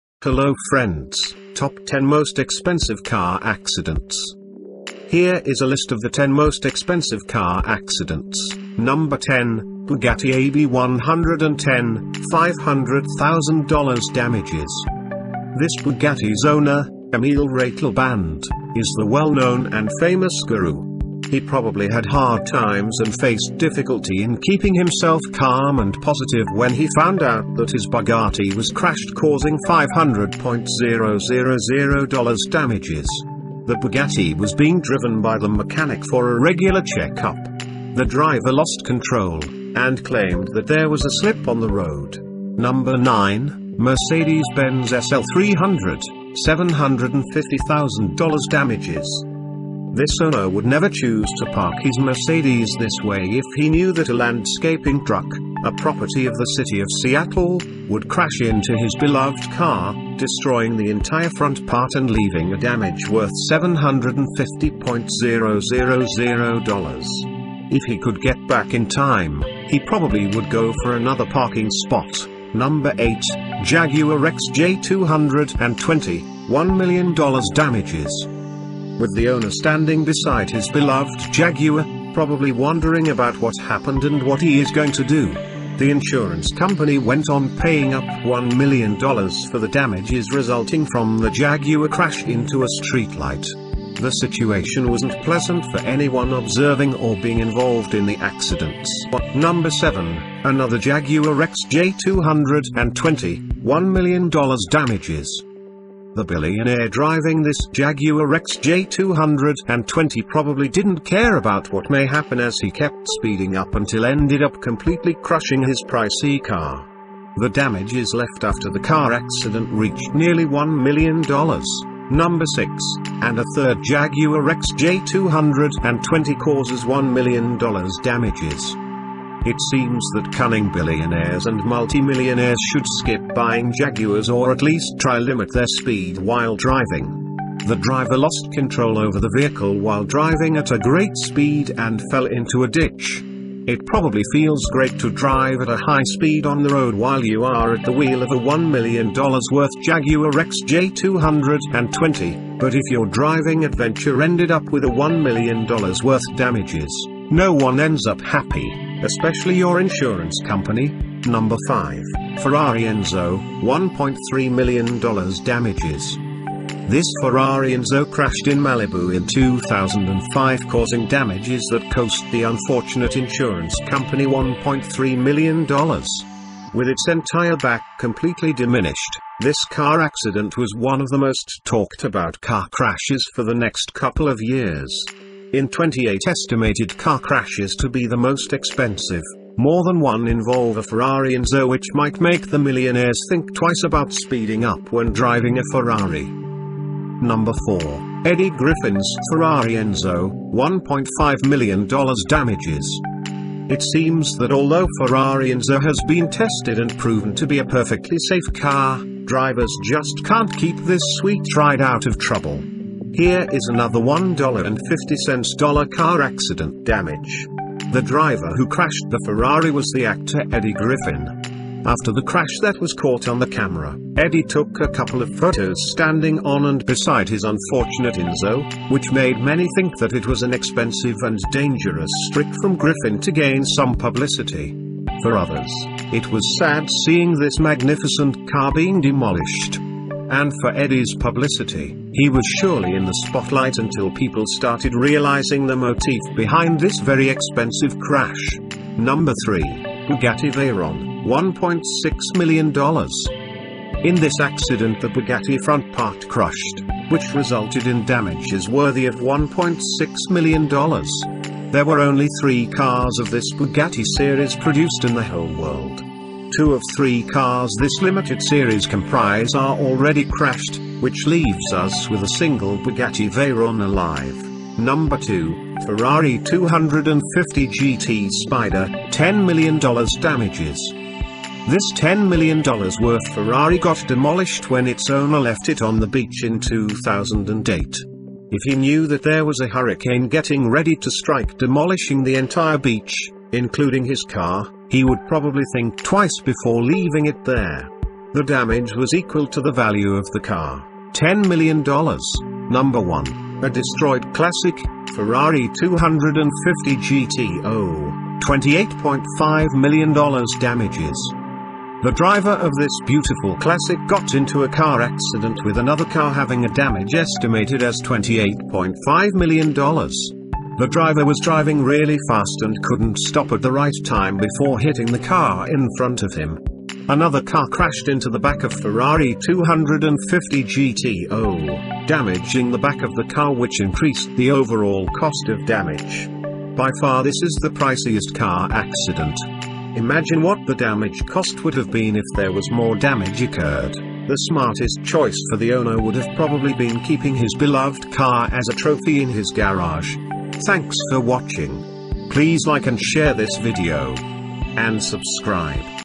Top 10 Most Expensive Car Accidents
10 Bugatti EB110 [500,000$ Damages]

This Bugatti’s owner, Emile Ratelband, is the well known and famous guru. He probably had hard times and faced difficulty in keeping himself calm and positive when he found out that his Buggati was crashed causing 500.000$ damages. The Bugatti was being driven by the mechanic for a regular check up. The driver lost control, and claimed that there was a slip on the road.

9 Mercedes Benz SL 300 [750,000$ Damages]

This owner would never choose to park his Mercedes this way if he knew that a landscaping truck, a property of the city of Seattle, would crash into his beloved car, destroying the entire front part and leaving a damage worth 750.000$. If he could get back in time, he probably would go for another parking spot.

8 Jaguar XJ220 [$1 Million Damages]

With the owner standing beside his beloved Jaguar, probably wondering about what happened and what he is going to do, the insurance company went on paying up 1 million dollars for the damages resulting from the Jaguar crash into a streetlight. The situation wasn’t pleasant for anyone observing or being involved in the accidents.

7 Another Jaguar XJ220 [1$ million Damages]

The billionaire driving this Jaguar XJ220 probably didn’t care about what may happen as he kept speeding up until ended up completely crushing his pricey car. The damages left after the car accident reached nearly 1$ million.

6 And a third Jaguar XJ220 causes 1$ million Damages

It seems that cunning billionaires and multi millionaires should skip buying Jaguars or at least try limit their speed while driving. The driver lost control over the vehicle while driving at a great speed and fell into a ditch. It probably feels great to drive at a high speed on the road while you are at the wheel of a 1$ million worth Jaguar XJ220, but if your driving adventure ended up with a 1$ million worth damages, no one ends up happy, especially your insurance company.

5 Ferrari Enzo [1.3$ million Damages]

This Ferrari Enzo crashed in Malibu in 2005 causing damages that coast the unfortunate insurance company  1.3$ million. With its entire back completely diminished, this car accident was one of the most talked about car crashes for the next couple of years. In 28 estimated car crashes to be the most expensive, more than one involve a Ferrari Enzo which might make the millionaires think twice about speeding up when driving a Ferrari.

4 Eddie Griffin’s Ferrari Enzo [1.5$ million Damages]

It seems that although Ferrari Enzo has been tested and proven to be a perfectly safe car, drivers just can’t keep this sweet ride out of trouble. Here is another 1.5$ dollar car accident damage. The driver who crashed the Ferrari was the actor Eddie Griffin. After the crash that was caught on the camera, Eddie took a couple of photos standing on and beside his unfortunate Enzo, which made many think that it was an expensive and dangerous trick from Griffin to gain some publicity. For others, it was sad seeing this magnificent car being demolished. And for Eddie’s publicity, he was surely in the spotlight until people started realizing the motif behind this very expensive crash.

3 Bugatti Veyron [$1.6 million]

In this accident the Bugatti front part crushed, which resulted in damages worthy of 1.6$ million. There were only three cars of this Bugatti series produced in the whole world. Two of three cars this limited series comprise are already crashed, which leaves us with a single Bugatti Veyron alive.

2 Ferrari 250 GT Spyder [$10 million Damages]

This 10$ million worth Ferrari got demolished when its owner left it on the beach in 2008. If he knew that there was a hurricane getting ready to strike demolishing the entire beach, including his car, he would probably think twice before leaving it there. The damage was equal to the value of the car; 10 million dollars.
1 A Destroyed Classic – Ferrari 250 GTO [$28.5 million Damages]

The driver of this beautiful classic got into a car accident with another car having a damage estimated as $28.5 million. The driver was driving really fast and couldn’t stop at the right time before hitting the car in front of him. Another car crashed into the back of Ferrari 250 GTO, damaging the back of the car which increased the overall cost of damage. By far this is the priciest car accident. Imagine what the damage cost would have been if there was more damage occurred; the smartest choice for the owner would have probably been keeping his beloved car as a trophy in his garage.